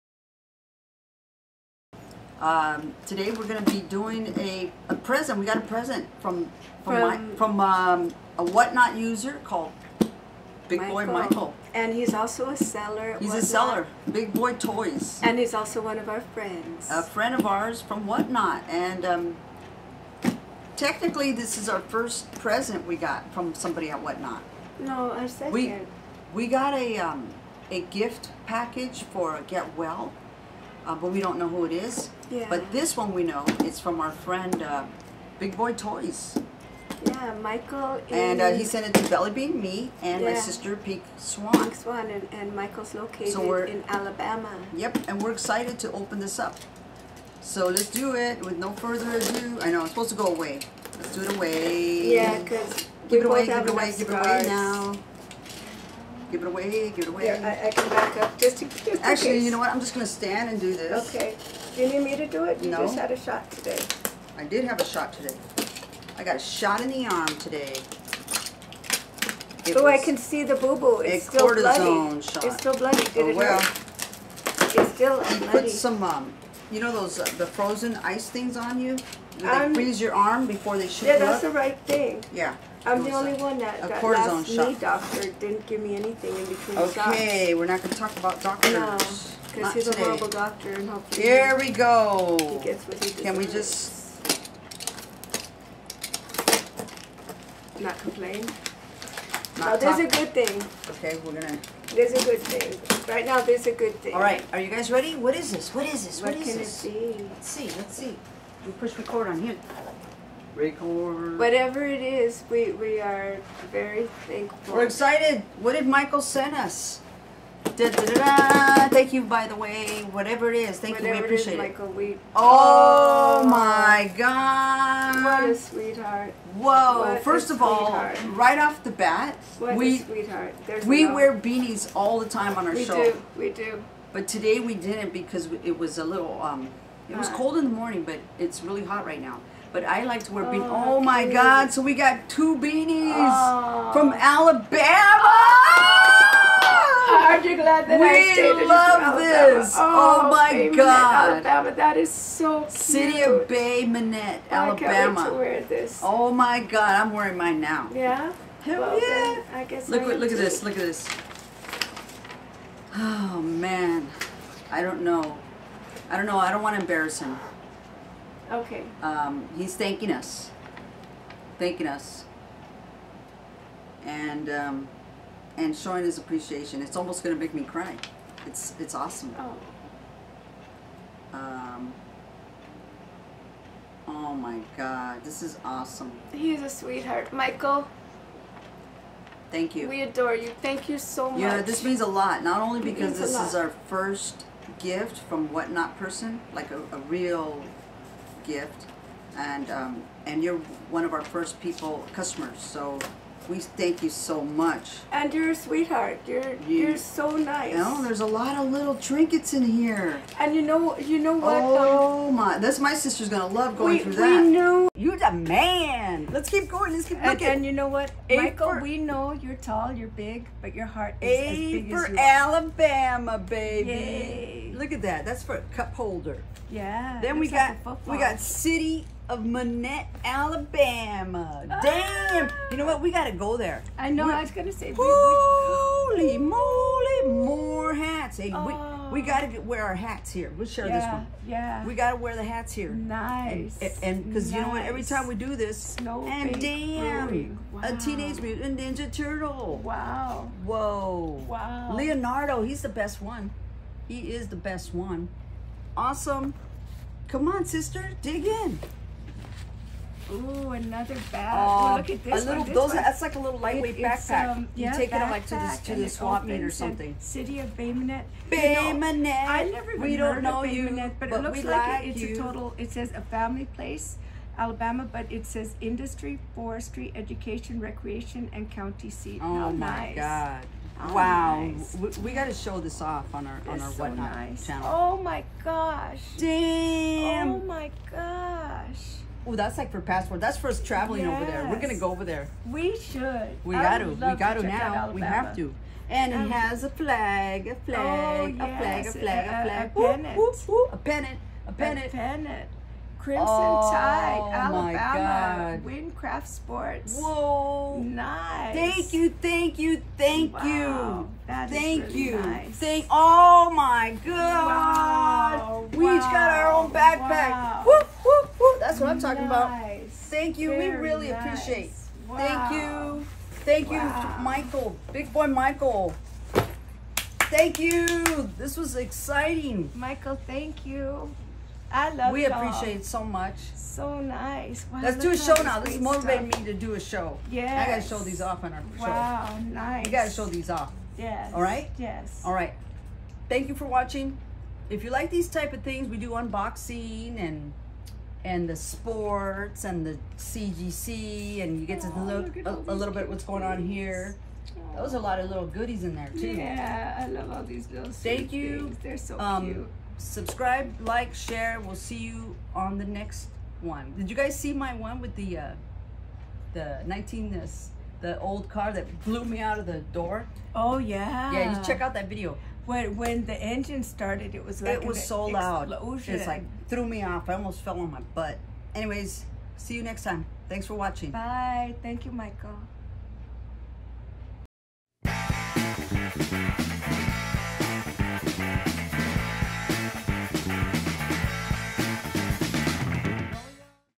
Bigboytoys sends us an Alabama care package
Totally unexpected arrives in the mail and it  turns out to be Alabama souvenirs and more